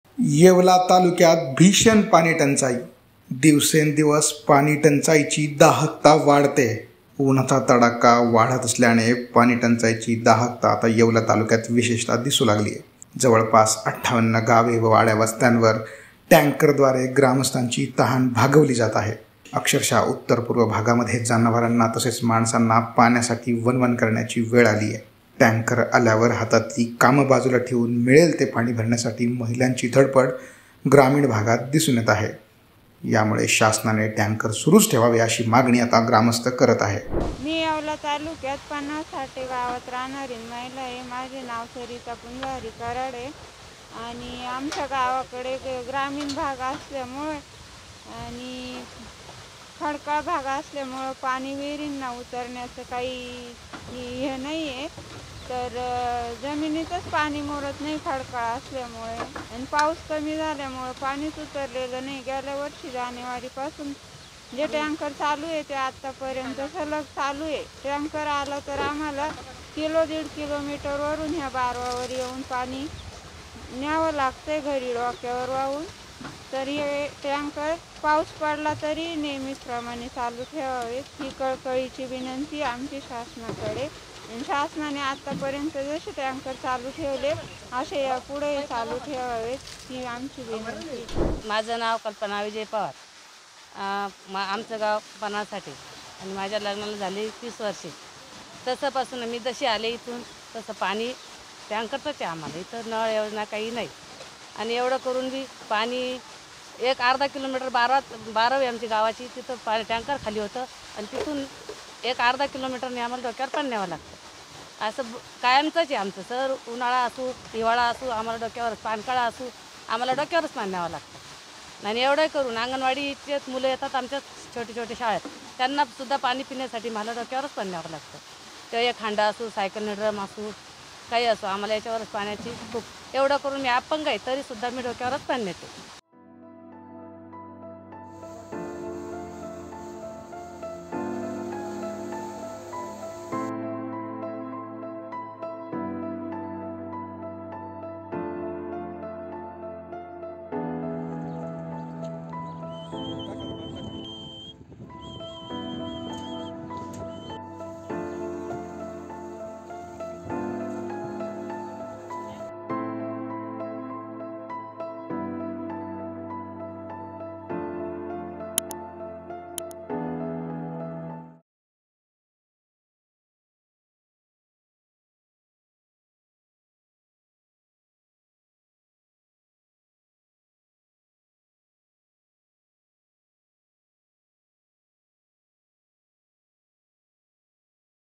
Nashik water Shortage : येवला तालुक्यात पाणीटंचाईचा उद्रेक... | LetsUpp Marathi
दिवसेंदिवस उन्हाचा तडाखा वाढत असल्याने पाणीटंचाईची दाहकता देखील वाढतांना येवला तालुक्यात दिसून येत आहे.